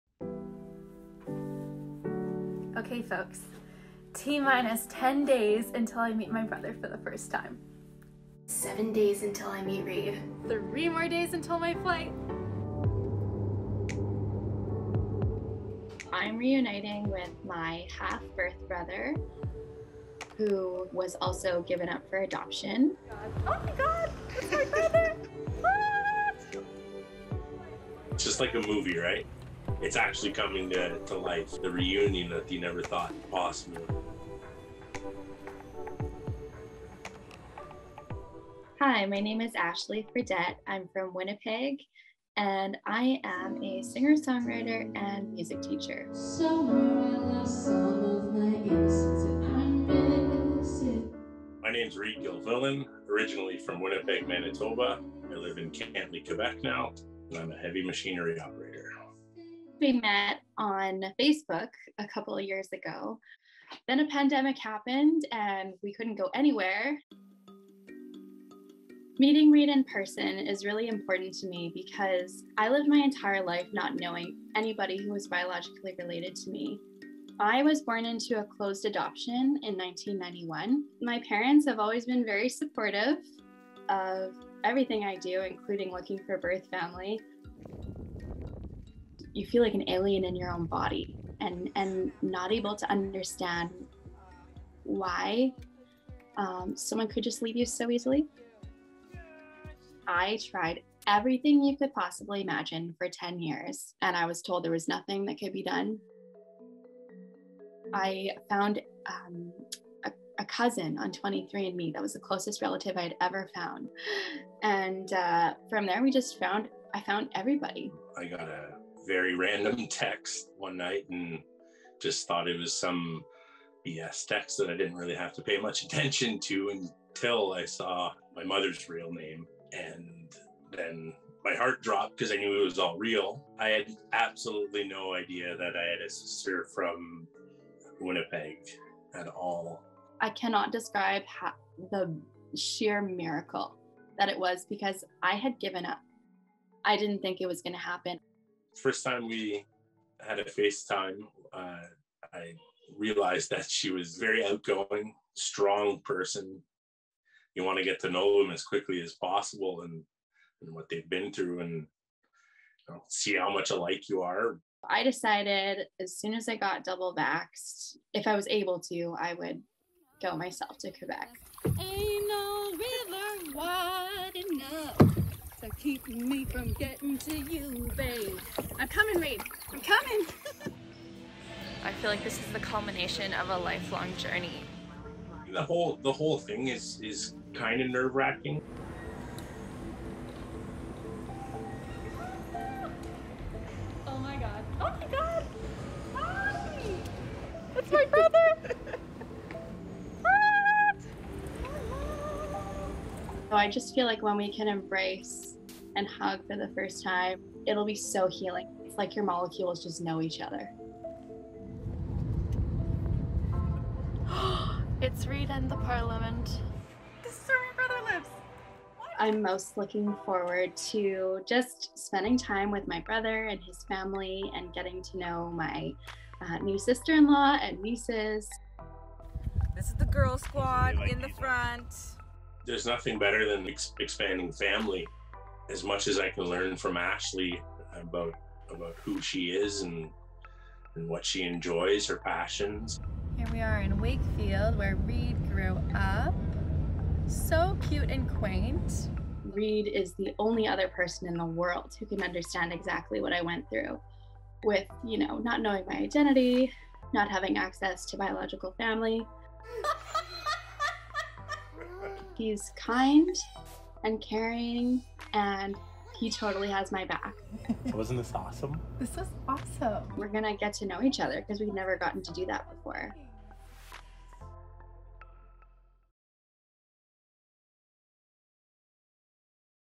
Adopted siblings finally meet after pandemic delay
Just before the COVID-10 pandemic, Ashley Fredette found her older brother Reid Gilfillan after years of searching for her biological family for almost a decade, but public health restrictions keep them from meeting in person, until now.